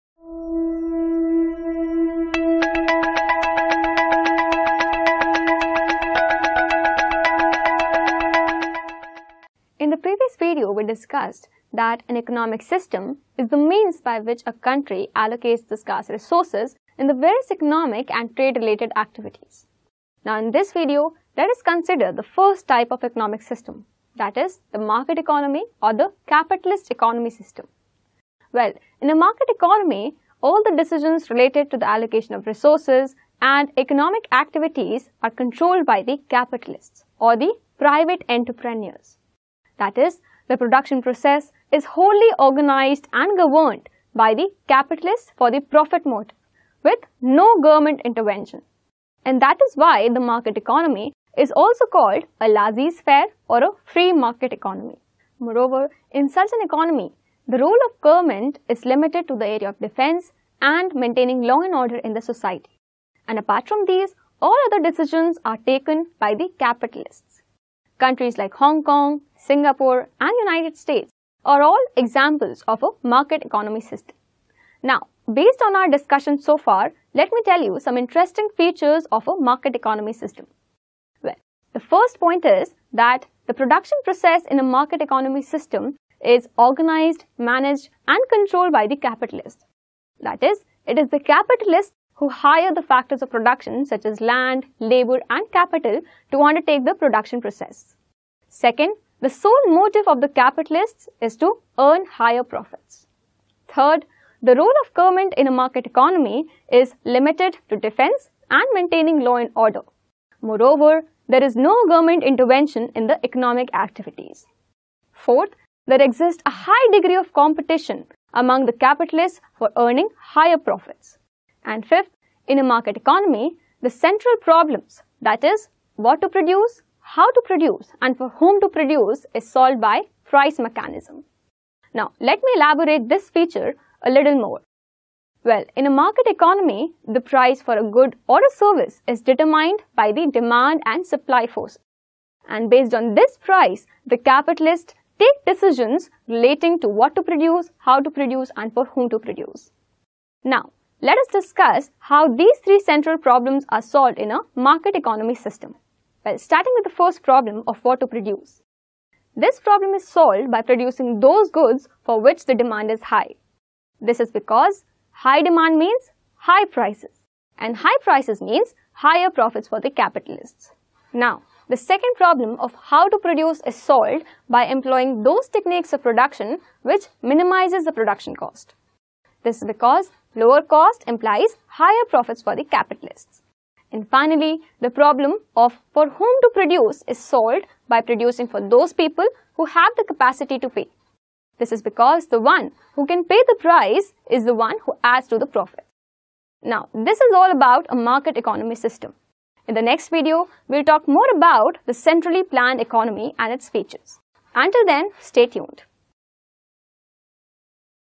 Economics fo Class 12: Market Capitalist Economy - Features and Solutions
Our Economics expert explains CBSE Class 12 topic Market Capitalist Economy - Features and Solutions, in this short and yet comprehensive video tutorial.

For more videos about topics in CBSE Class 6 -12 Maths and Science,